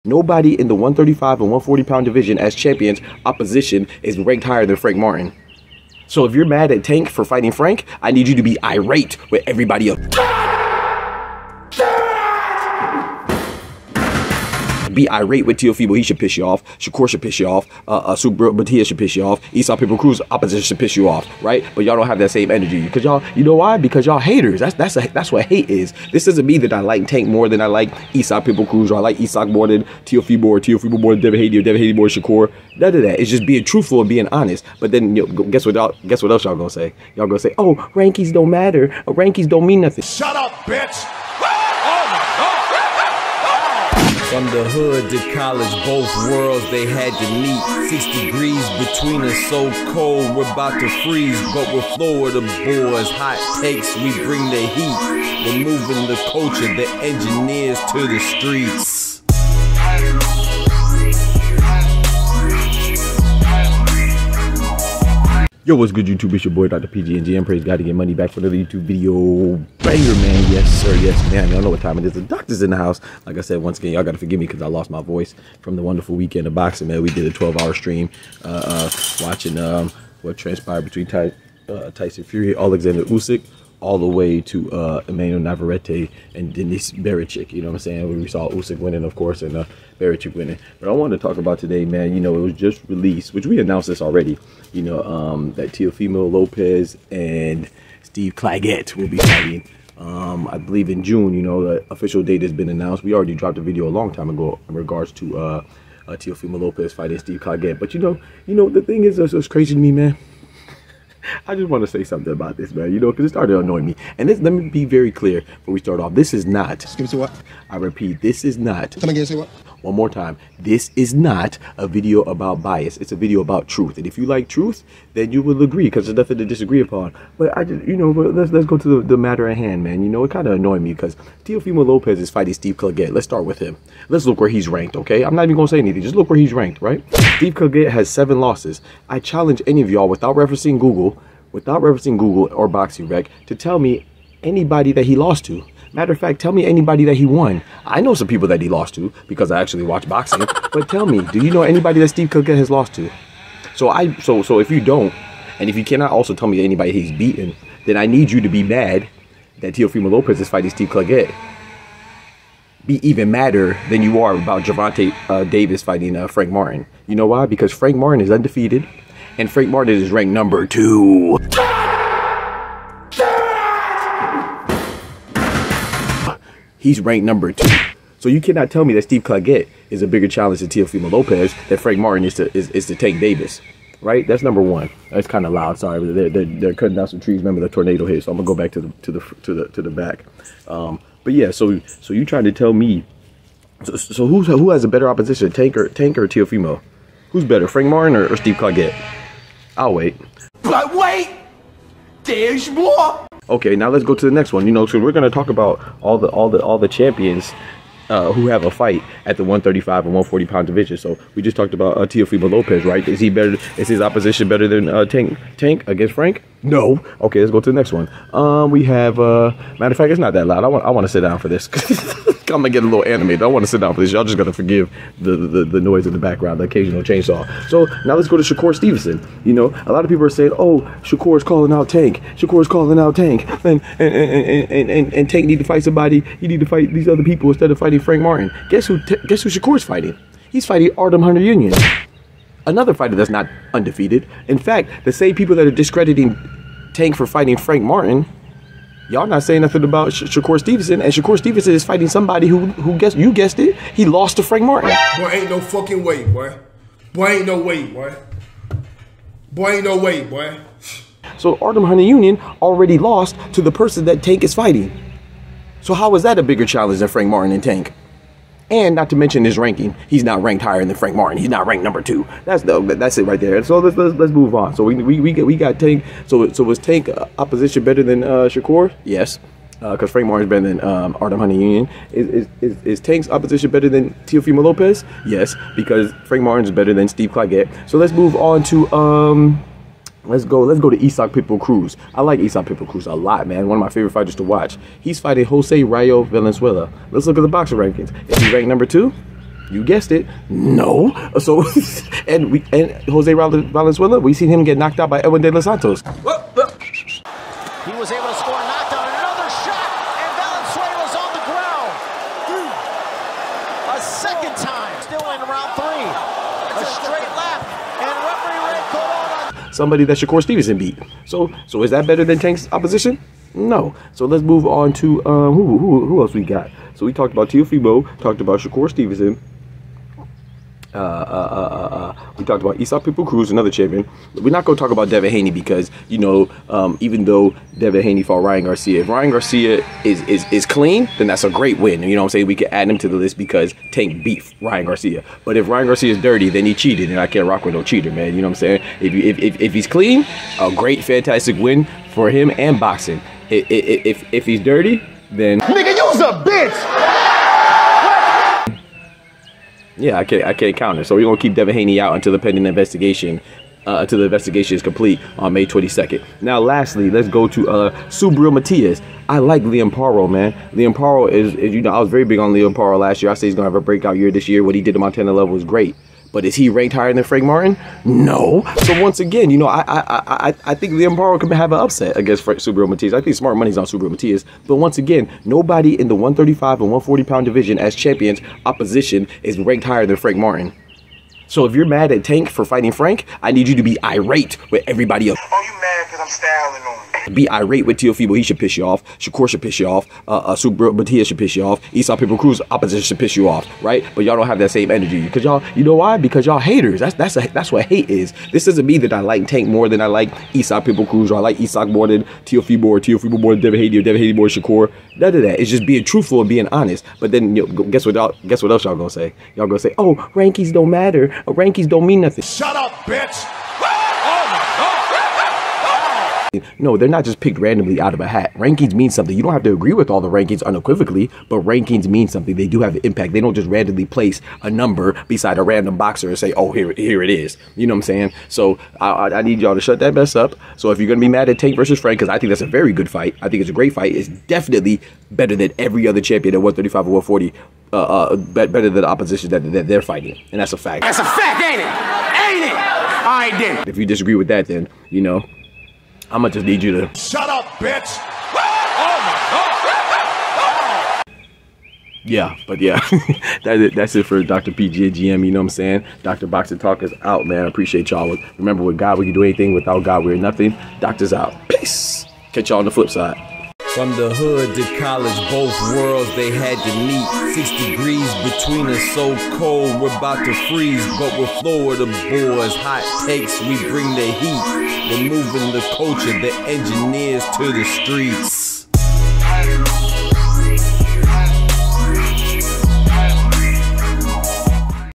BREAKING NEWS: Teofimo Fight Date Revealed! Gervonta Takes on TOUGHEST COMPETITION in Division
Teofimo Lopez next opponent announced, he's fighting Steve Claggett... People are mad, right? I thought Gervonta Tank Davis is cherry picking Frank Martin, but NOBODY knows the fighters that Shakur and Teofimo are fighting! Doc discusses.

Teofimo Lopez vs Steve Claggett fight date announced! Gervonta Davis vs Frank Martin is Better